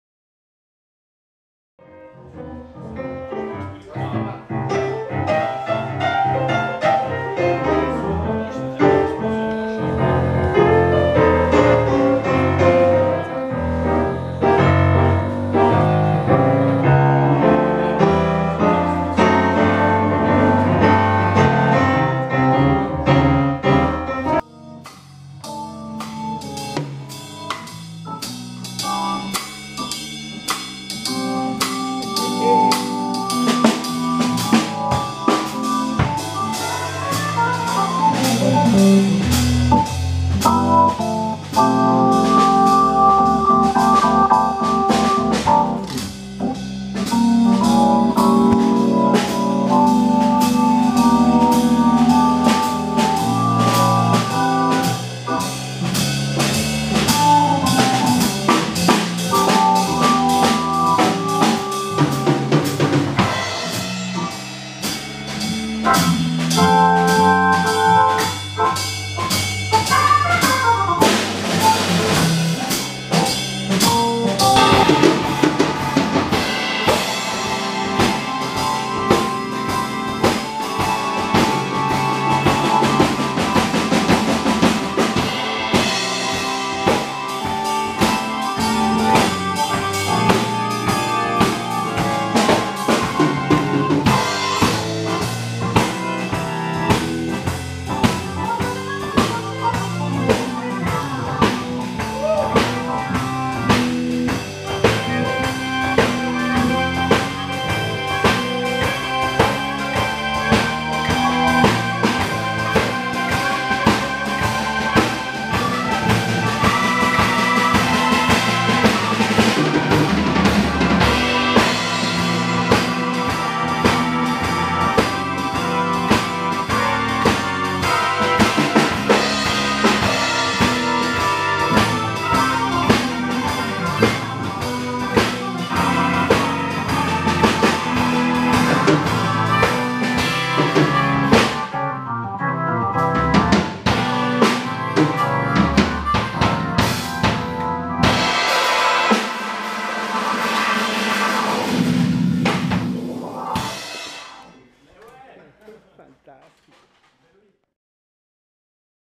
DAMIEN CORNELIS and ENRICO MATTIOLI at the ROYAL EST MUSIC 18 SEPT 2018 FINCA EL REGOCIJO
An improvised and unexpected jam session of these two excellent musicians
UNA OPORTUNIDAD UNICA APROVECHADA POR JUANITO GUANABACOA...